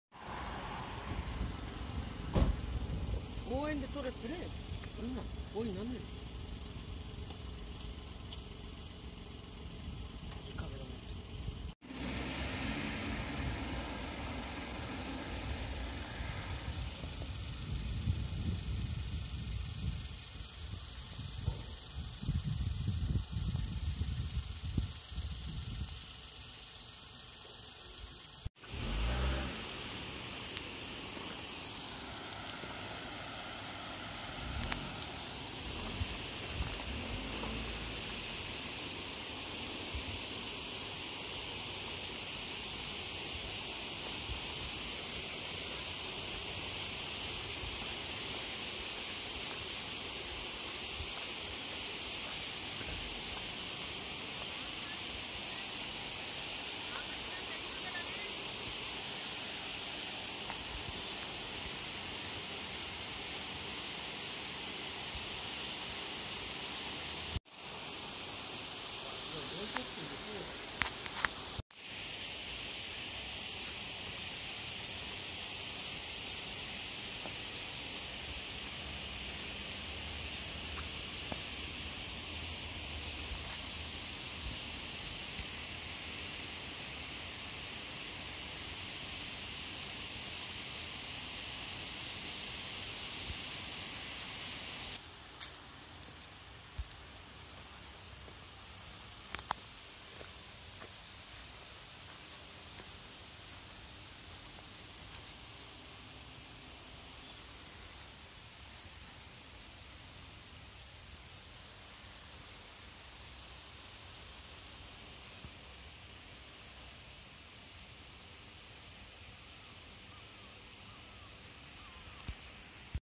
只見線不通区間の現状を調査 2018年8月19日
橋も倒壊してるままですね